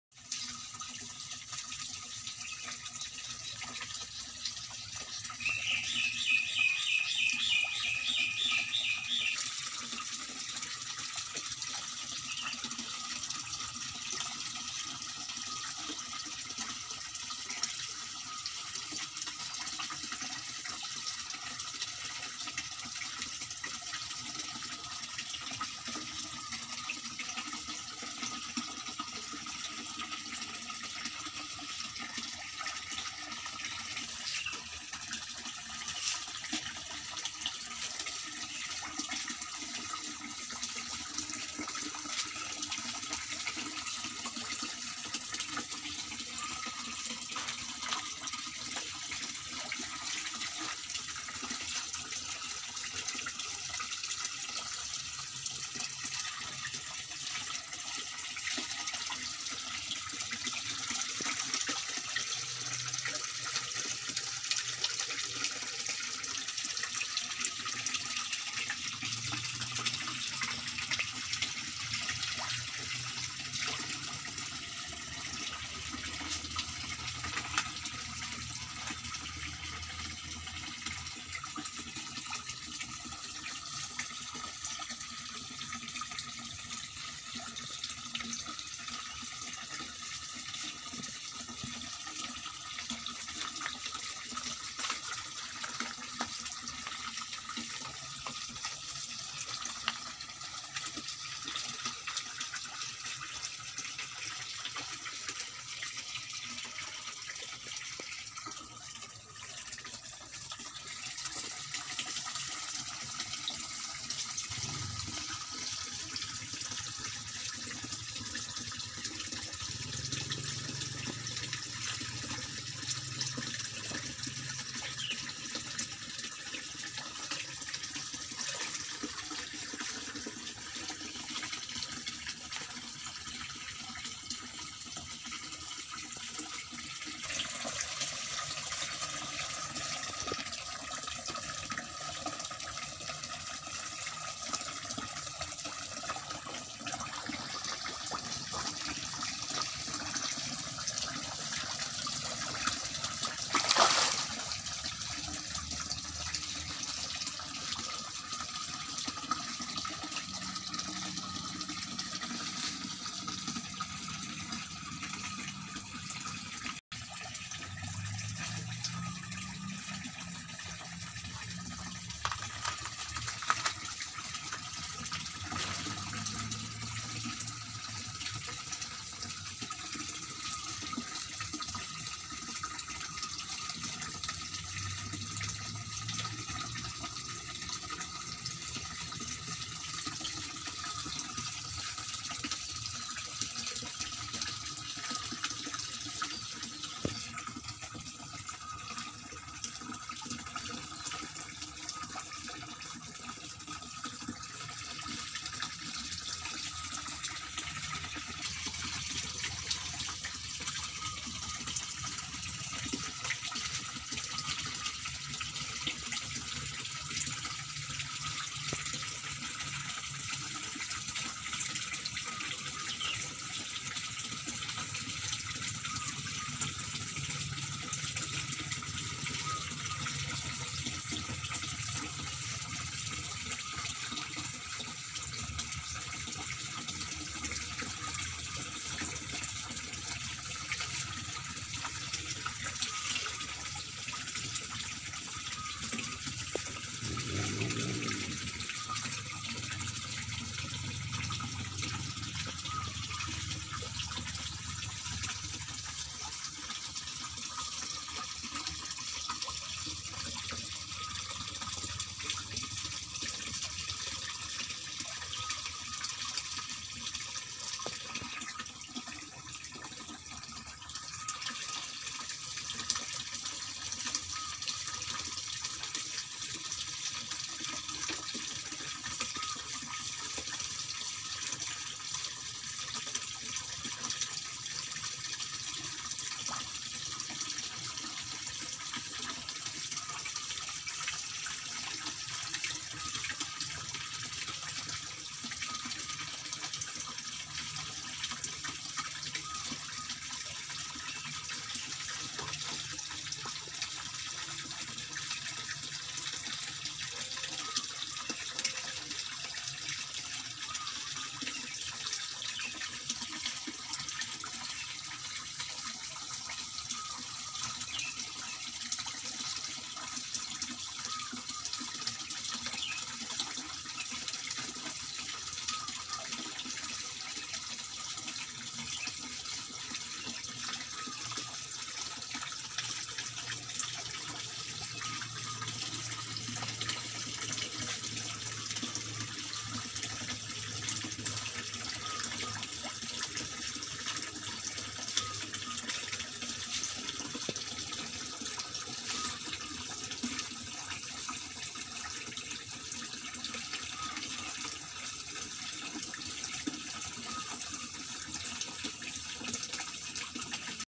Kolam Ikan Koi Minimalis Balkon Belakang Rumah
Assalam'ualaikum Wr. Wb.
Bagaimana kabarnya kalian semua, semoga selalu dalam lindungannya 🙏
Video kali ini, saya memperlihatkan Kolam Ikan Koi milik Cak Hadi, di sini Kolam Ikan Koi berukuran Panjang 2m x Lebar 1,5m dengan kedalaman kurang lebih 50cm. Kolam Koi ini sangat bagus, karena dengan pemandangan Alam asri yang mengagumkan 😍

jangan lupa buat yang belum Subscriber segera Subscriber, untuk membantu perkembangan Channel ini 👍